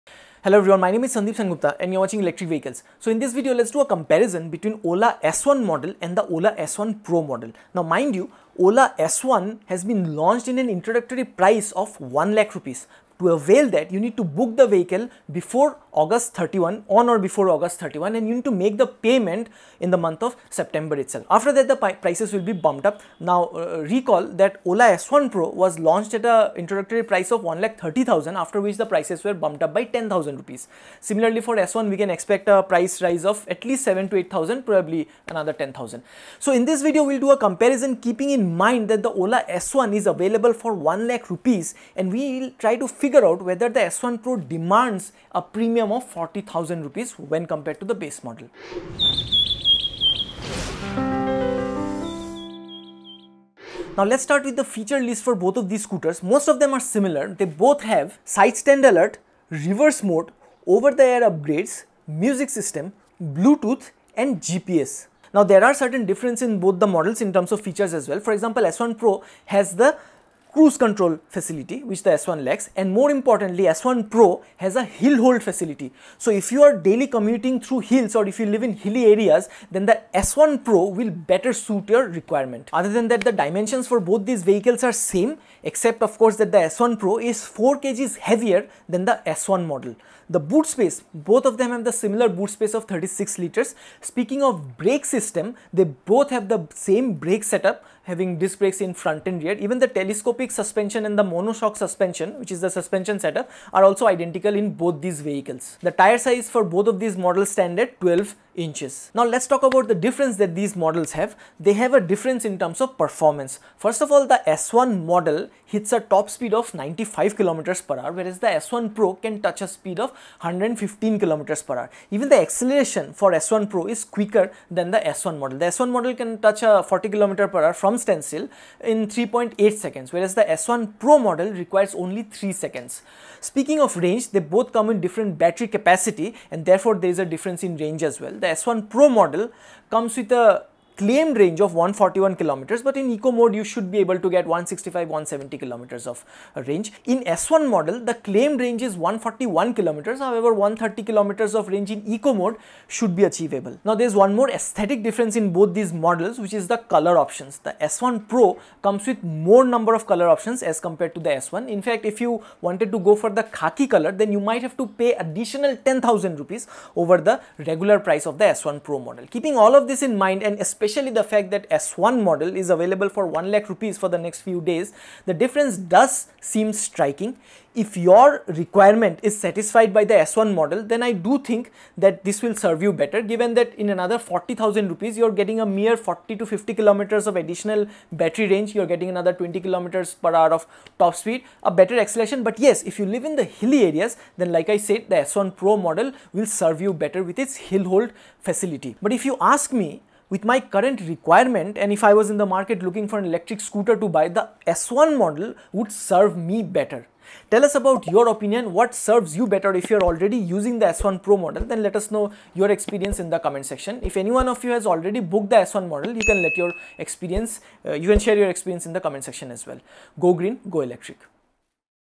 OLA S1 Pro VS OLA S1 | OLA S1 Getting For Lowest Ever Price | Electric Scooters India | EV India
👉OLA S1  as of August 2022
Acceleration(0-40 km/h)  3.8sec
Home Charging Time 5 hr
Motor Power (Nominal/Peak)  5.5kw /8.5kw

Boot Space  36L
True Range  128km

👉OLA S1 PRO as of August 2022
Top Speed : 116kmph
Range: 181km
Acceleration(0-40 km/h)  2.9 sec
Home Charging Time 6 hr 30 min
Boot Space  36L
True Range  170km